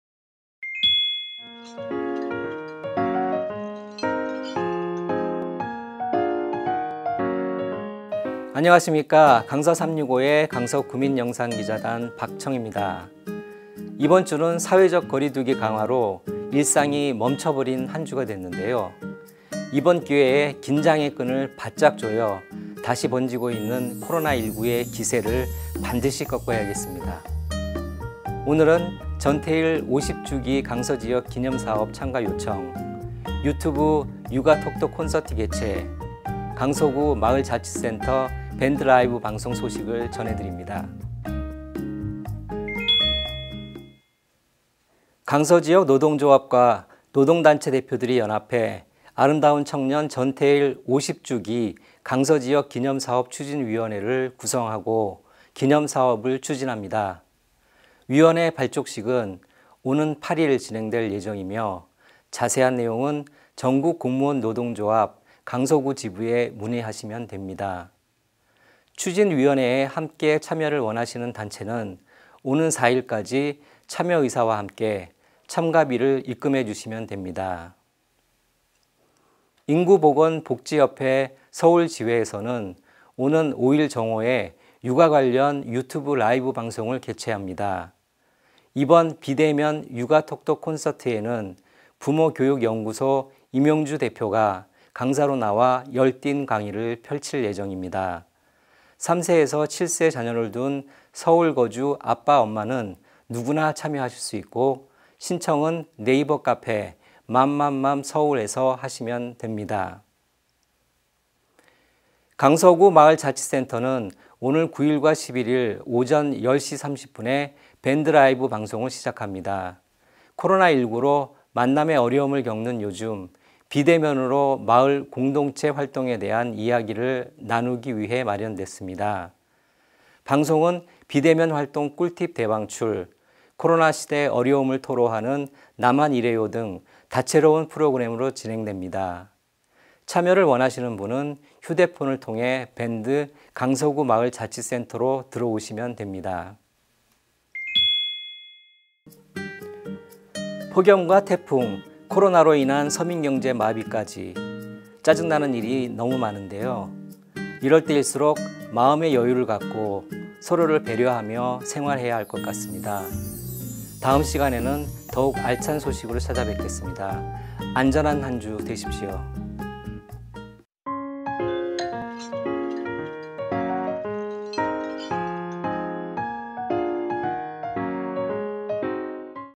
《강서365》 14회 _ 전태일 50주기 강서지역 기념사업, 육아 톡톡 콘서트, 강서구 마을자치센터 밴드 라이브
“강서365”에 강서구민영상기자단 박청입니다.

이번 주는 사회적 거리 두기 강화로 일상이 멈춰버린 한주가 됐는데요.
이번 기회에 긴장의 끈을 바짝 조여 다시 번지고 있는 코로나 19의 기세를 반드시 꺾어야겠습니다.

오늘은 전태일 50주기 강서지역 기념사업 참가 요청, 유튜브 「육아 톡톡 콘서트」 개최, 강서구 마을자치센터 밴드 라이브 방송 소식을 전해드립니다.

강서지역 노동조합과 노동단체 대표들이 연합해 아름다운 청년 전태일 50주기 강서지역 기념사업 추진위원회를 구성하고 기념사업을 추진합니다.

위원회 발족식은 오는 8일 진행될 예정이며 자세한 내용은 전국 공무원 노동조합 강서구 지부에 문의하시면 됩니다. 추진위원회에 함께 참여를 원하시는 단체는 오는 4일까지 참여의사와 함께, 참가비를 입금해주시면 됩니다.

인구보건 복지협회 서울지회에서는 오는 5일 정오에 육아 관련 유튜브 라이브 방송을 개최합니다.

이번 비대면 육아 톡톡 콘서트에는 부모교육연구소 임영주 대표가 강사로 나와 열띤 강의를 펼칠 예정입니다. 3세에서 7세 자녀를 둔 서울 거주 아빠ㆍ엄마는 누구나 참여하실 수 있고 청은 네이버 카페 「맘맘맘서울」에서 하시면 됩니다.

강서구 마을자치센터는 오는 9일과 11일 오전 10시 30분에 밴드 라이브 방송을 시작합니다.

코로나 19로 만남에 어려움을 겪는 요즘, 비대면으로 마을공동체 활동에 대한 이야기를 나누기 위해 마련됐습니다. 방송은 비대면 활동 꿀팁 대방출, 코로나 시대 어려움을 토로하는 '나만 이래요?' 등 다채로운 프로그램으로 진행됩니다.  참여를 원하시는 분은 휴대폰을 통해 밴드 ‘강서구 마을 자치센터’로 들어오시면 됩니다.

폭염과 태풍, 코로나로 인한 서민경제 마비까지 짜증 나는 일이 너무 많은데요. 이럴 때일수록 마음의 여유를 갖고 서로를 배려하며 생활해야 할 것 같습니다.

다음 시간에는 더욱 알찬 소식으로 찾아뵙겠습니다.
안전한 한 주 되십시오.